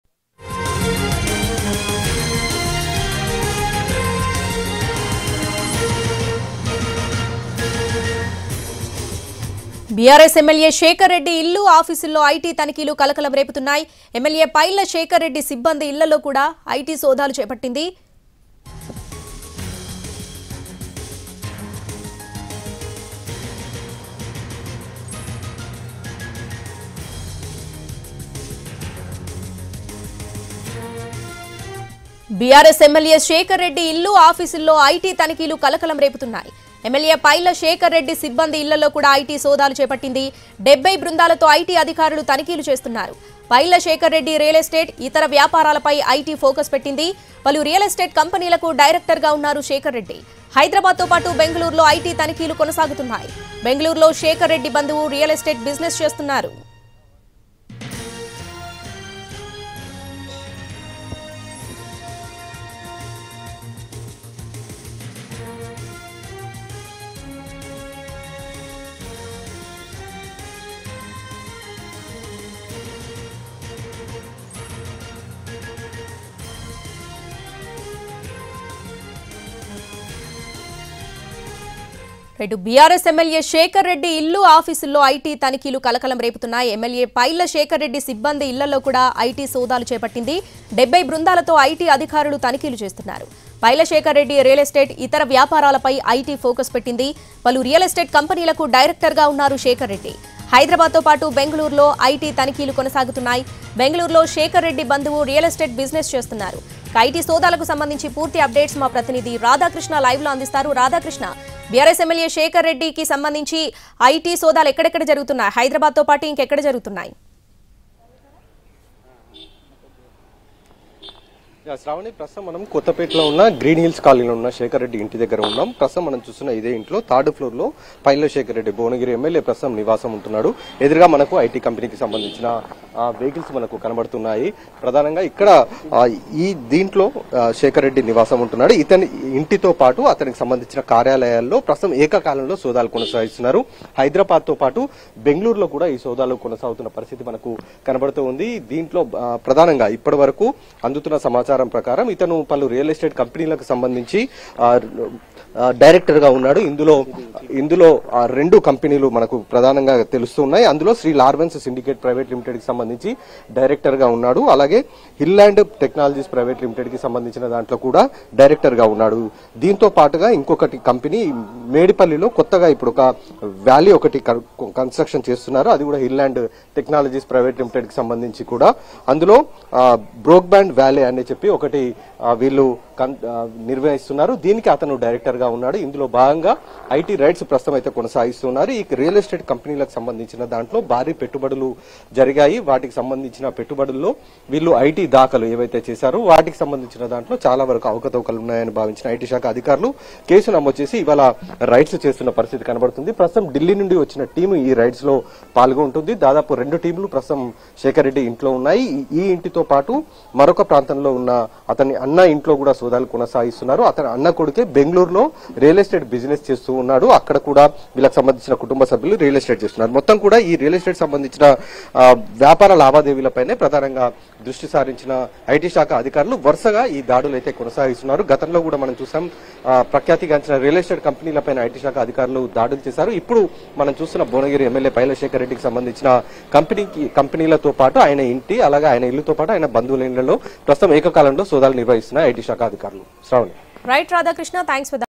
Live Report over IT raids on BRS MLA Pailla Shekar Reddy House | Ntv
Live Report over IT raids on BRS MLA Pailla Shekar Reddy | Ntv